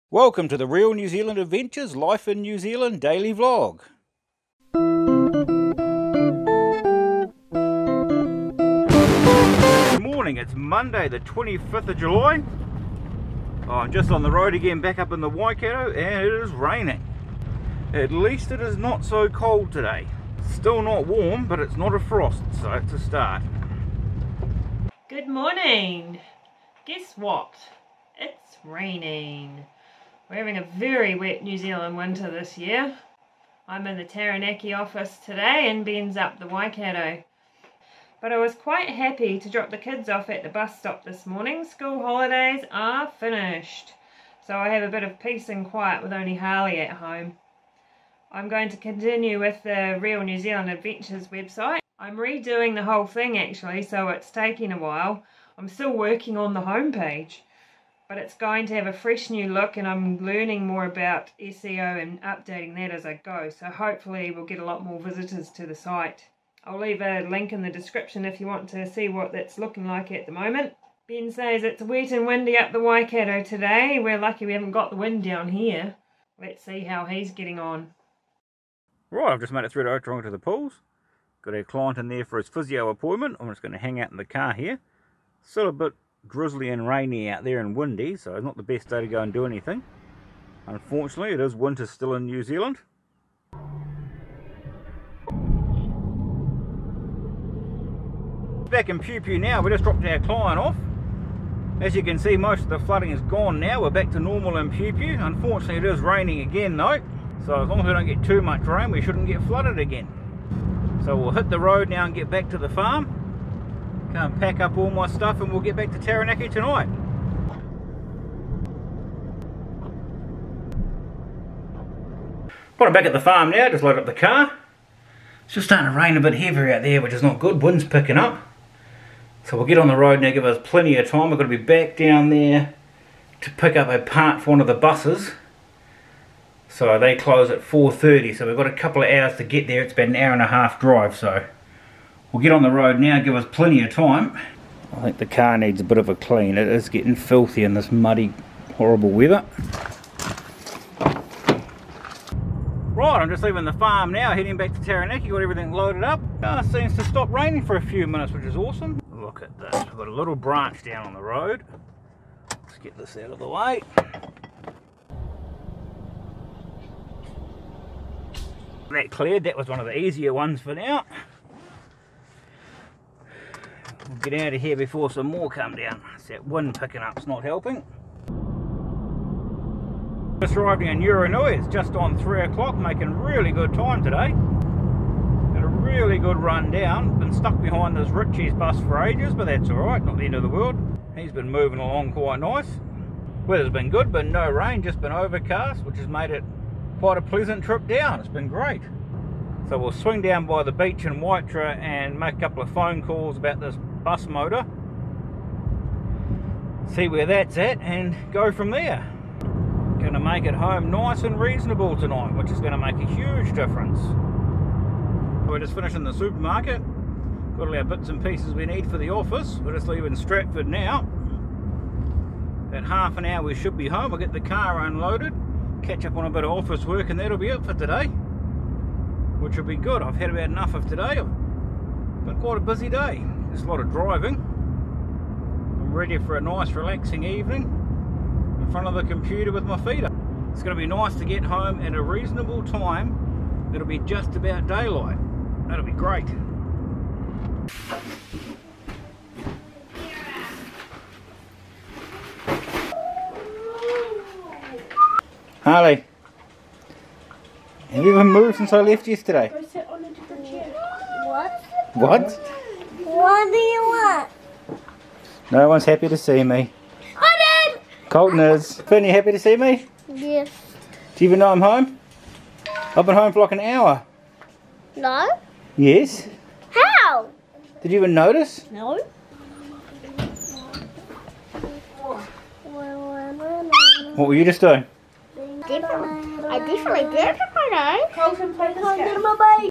Life in New Zealand Vlog #17 - Driving NZ roads in Winter
There's all sorts of weather conditions with the wind and rain as Benn travels around t Waikato today. Muddy roads and wind causing some debris to fall which he has to stop and pick up.  He gets a shot of the beach in Waitara on his way home to the Taranaki base while he stops in to pick up a part for a bus.

I'm loving the peace and quiet at home today since it's the first day back at school after the holidays.  The weather is another story! Typical winter, but it's been a very wet one.  I get some more website editing done, re-doing the whole Real New Zealand Adventures website and learning how to do the SEO as I go.  (this will be the previous version as I haven't published the updates yet - 25/7/22)

The rain has well and truly set in so we settle in for a cosy night in front of the fire.

In this episode you will see:
Driving in New Zealand, driving from Waikato to Taranaki, Waitara beach, driving in the rain, Patient Transport Service Waikato-Taranaki, Winter in New Zealand.